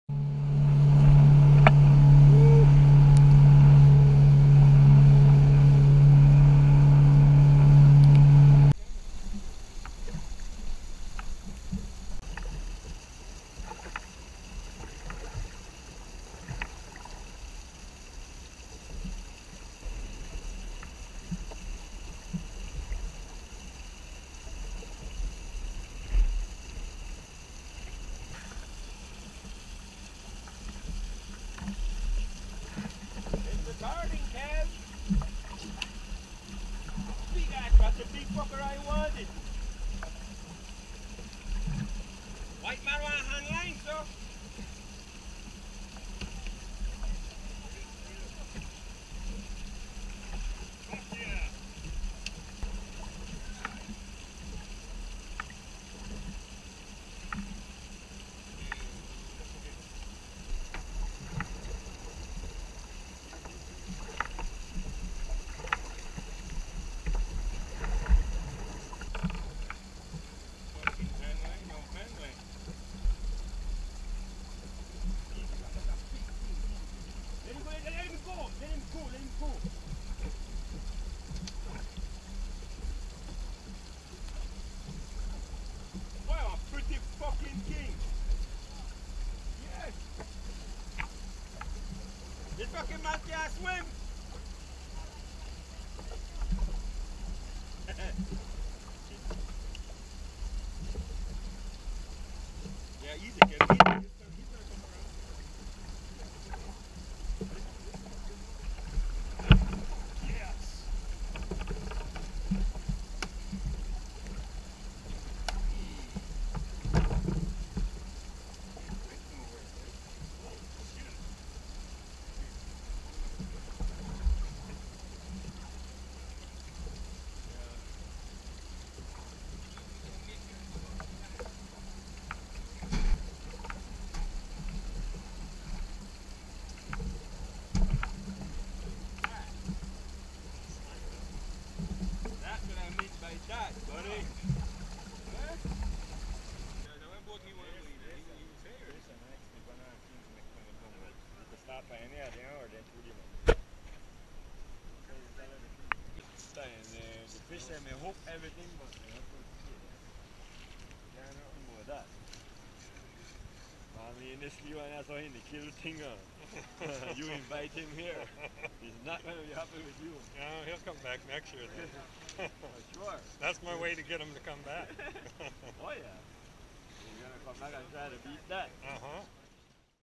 Belize handlining
Caught numerous fish handlining including a 20lb Barracuda and a 35lb
King mackerel.
Put some bait on a spool of 100lb mono, throw it in the ocean and hang
on!